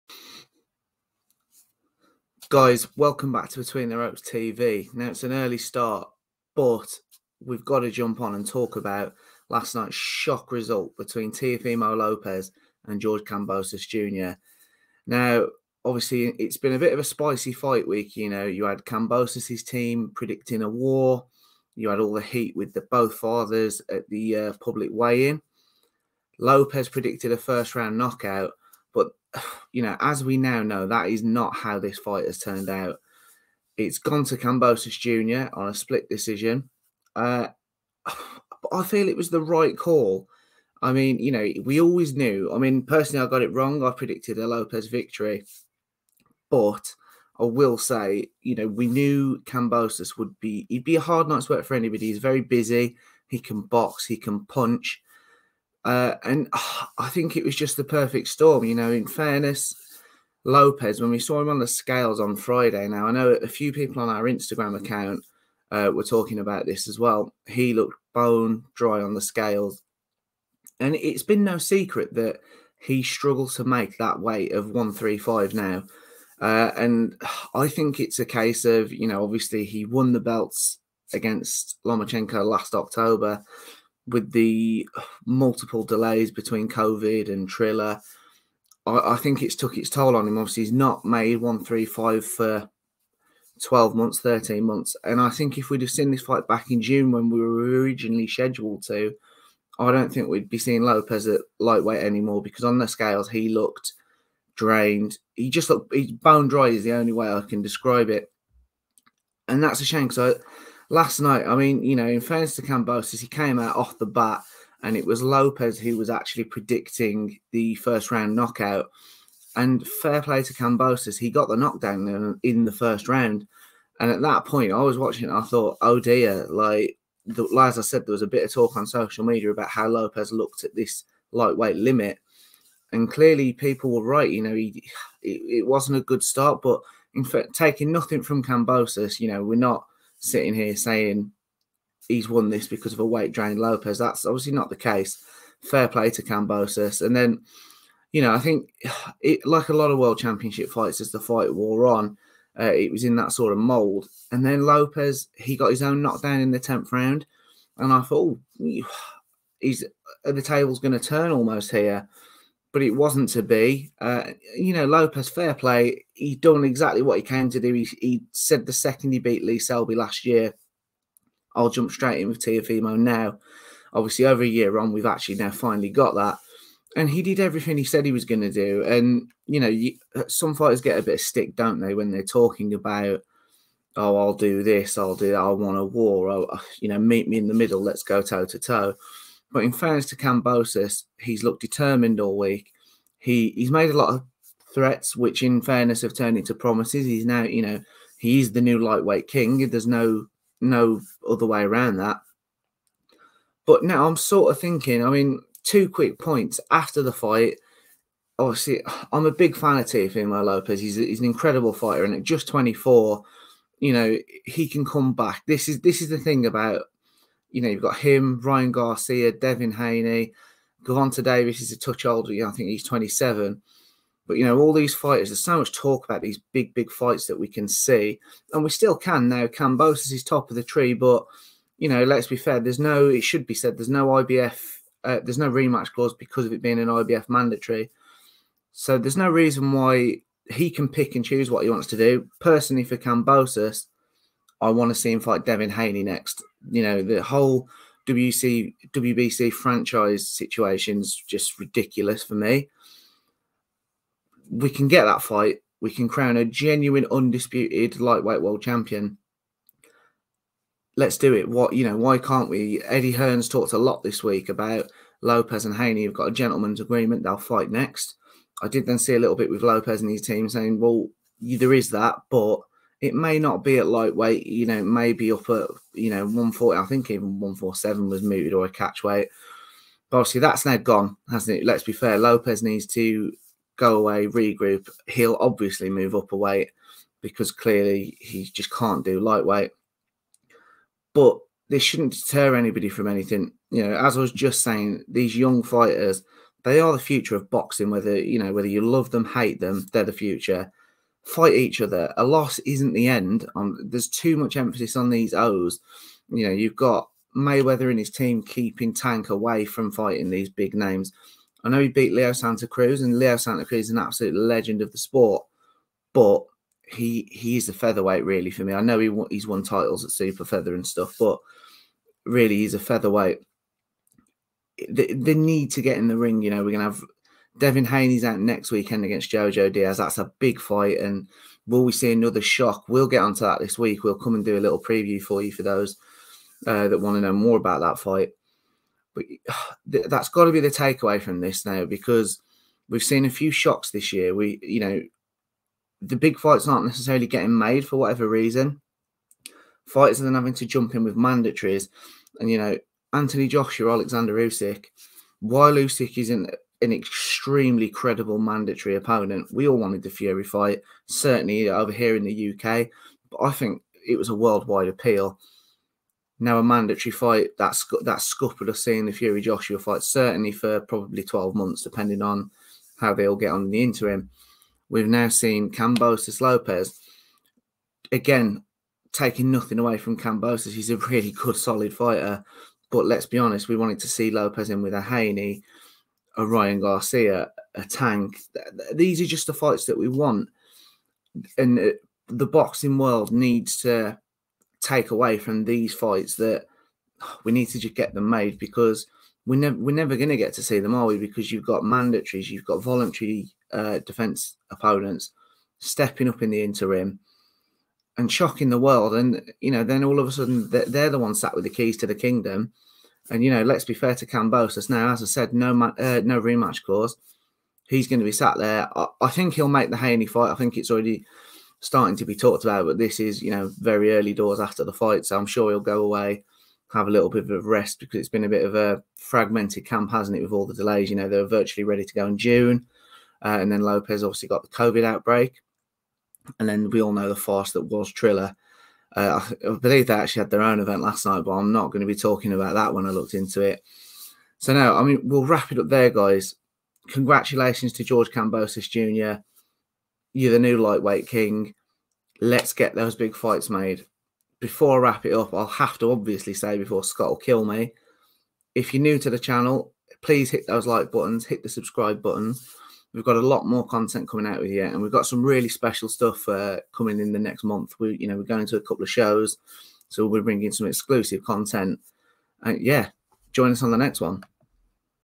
GEORGE KAMBOSOS JR STUNS TEOFIMO LOPEZ AT THE GARDEN TO BE CROWNED NEW LIGHTWEIGHT KING!!
George Kambosos Jr beats Teofimo Lopez to be crowned the New Lightweight King! In a great fight, they both traded knockdowns & this one fulfilled all the Fight Week hype! What now for Kambosos? Haney? Tank? Also, Lopez, will he now go on & try to become a two weight World Champion?!